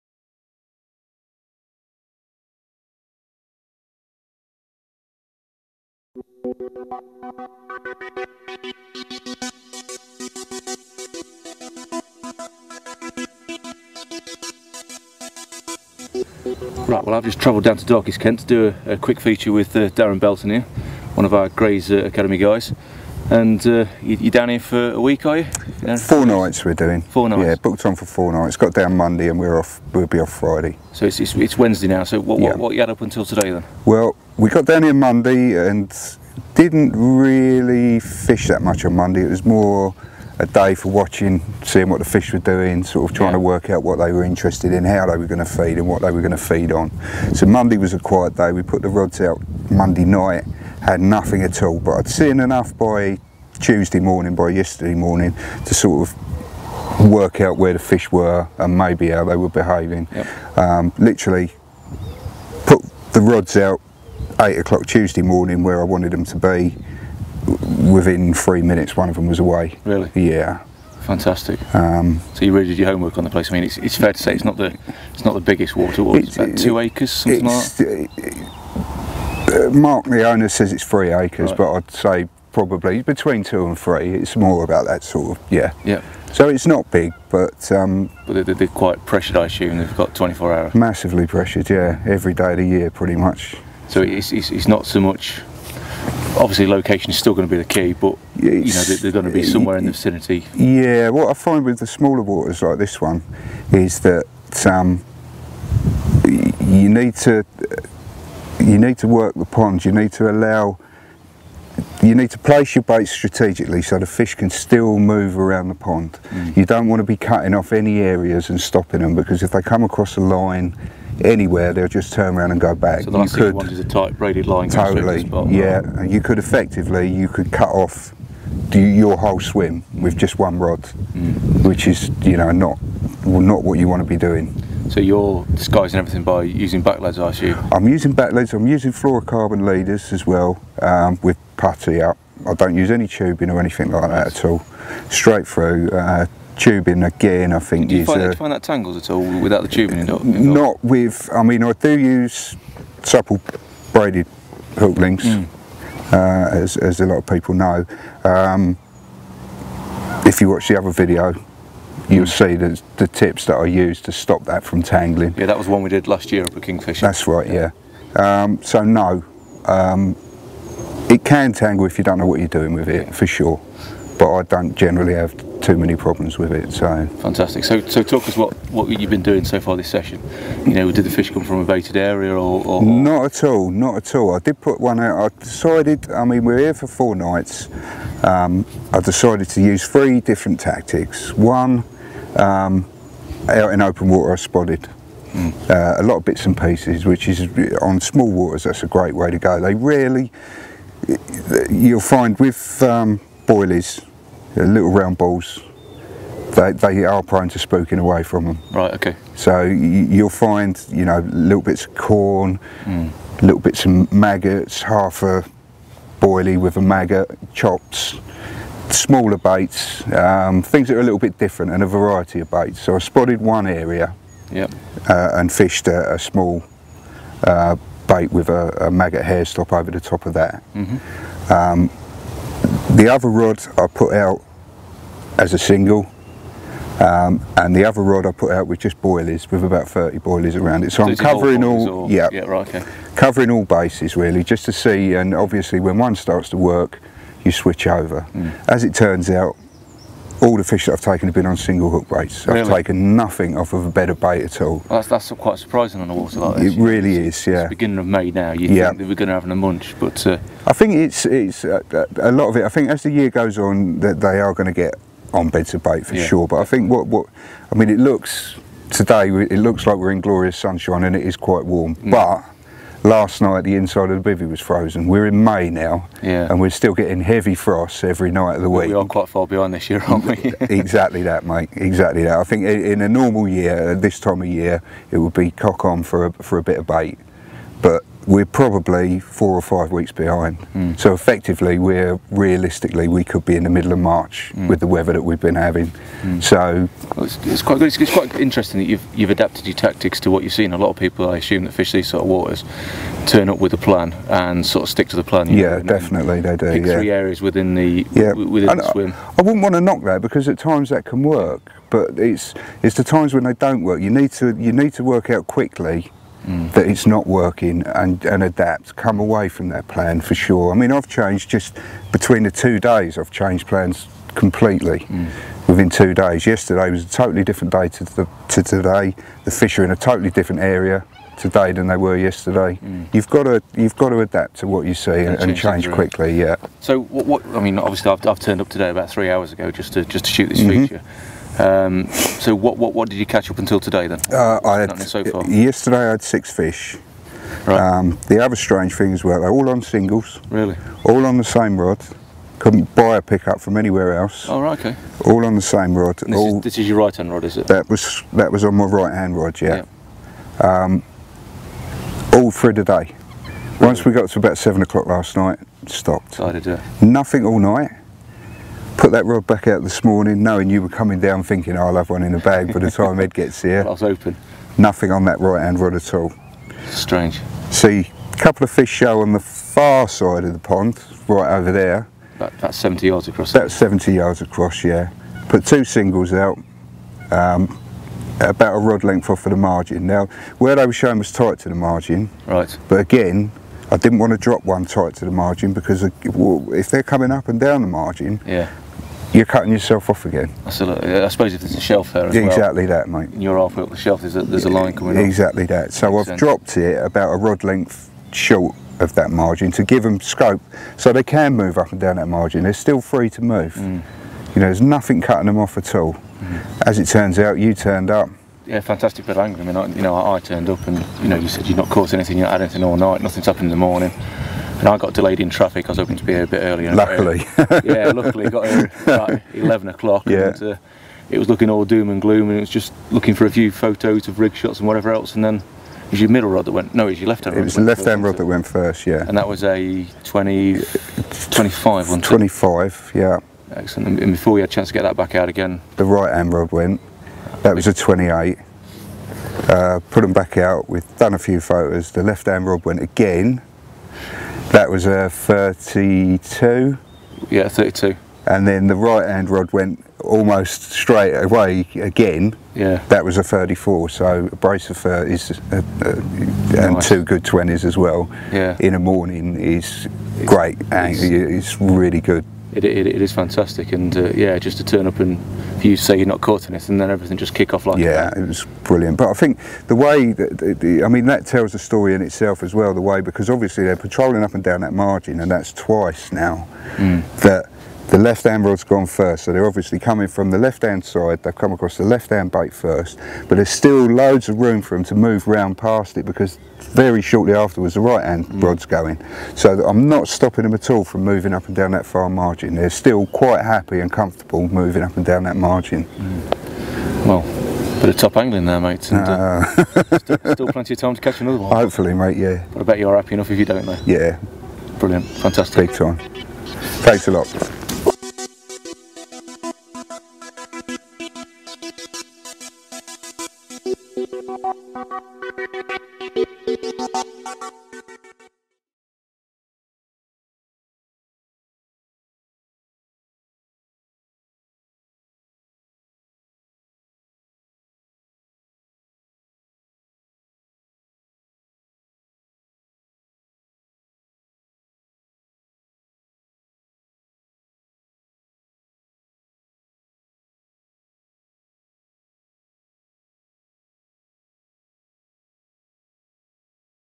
BIG CARP from small waters - top tactics
UKCarp magazine joins Greys & Chub carping duo Darren Belton and Ed Betteridge as they show you the tactics you need to catch big carp from small day ticket waters. With fish to 32lb can you afford not to try their tricks? Read UKCarp magazine every month - exclusively inside Angling Times.